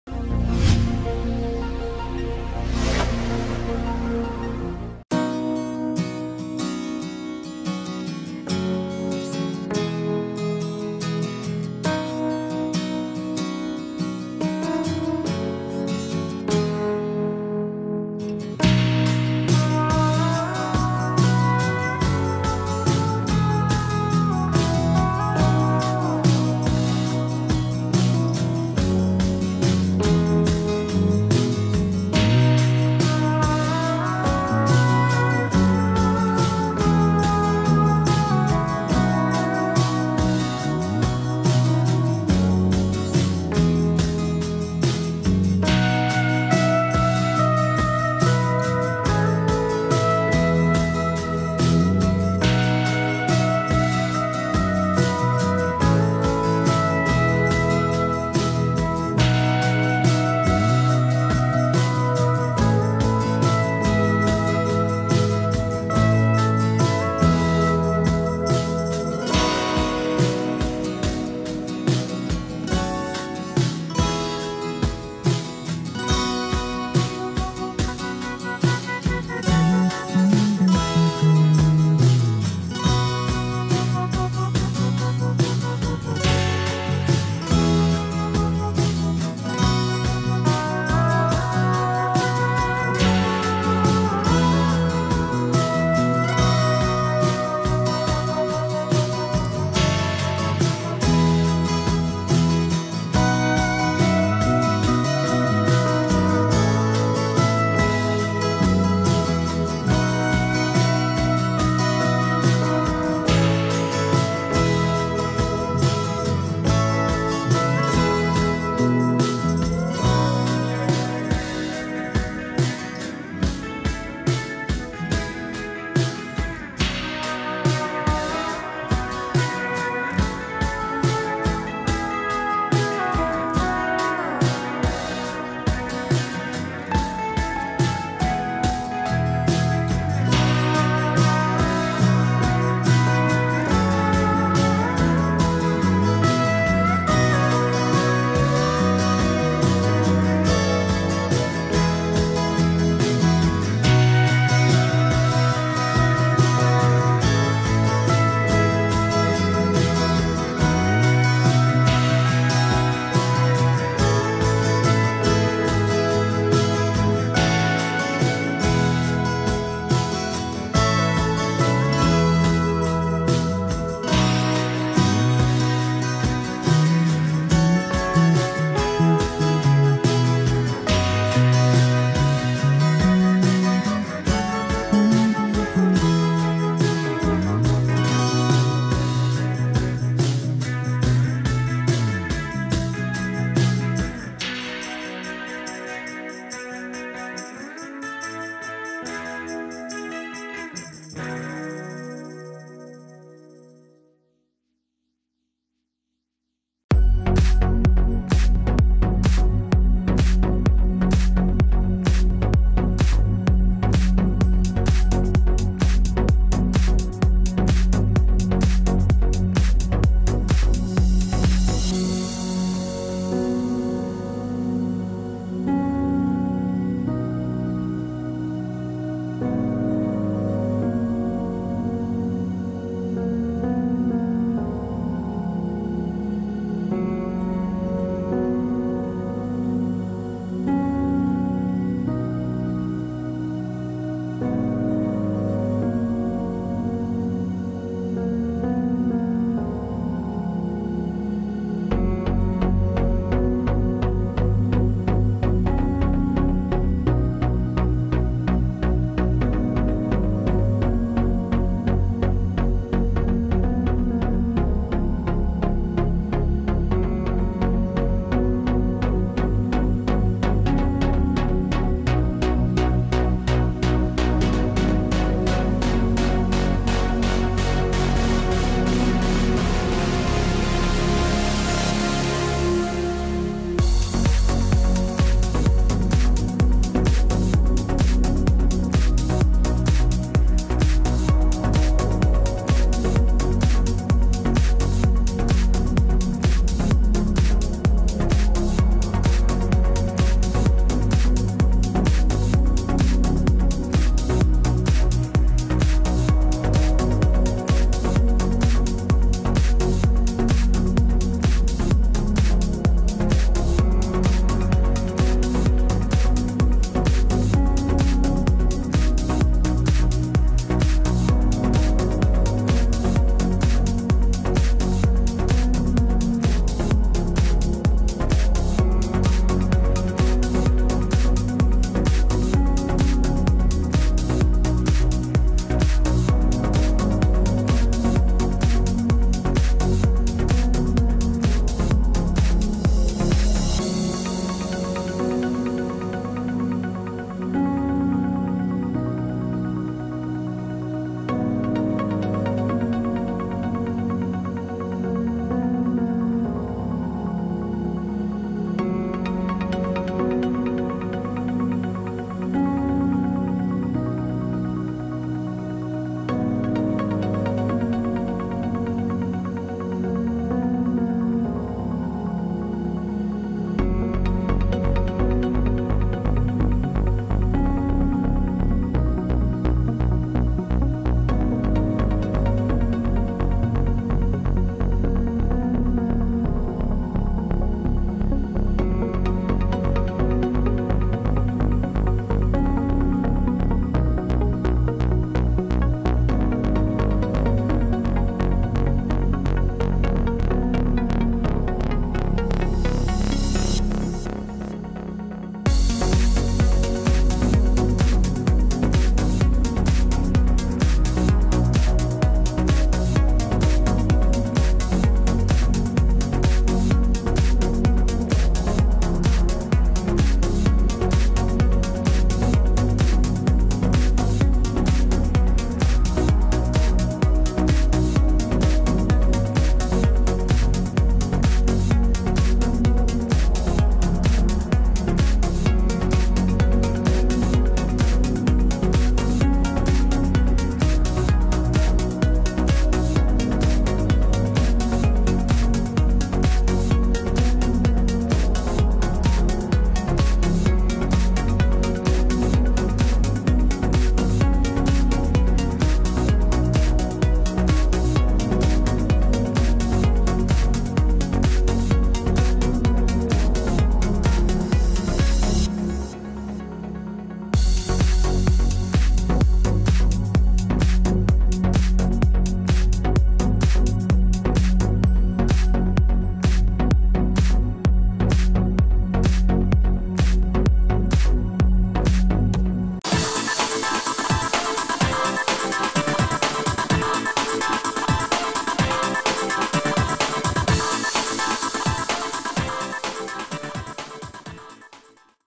Making of Durga Mata Idol at Dhoolpet | Durga Murti Making 2019 in Hyderabad
Making of Eyes Blinking Durga Mata Idol in Dhoolpet at Lakshmi Narayan Kalakar Workshop.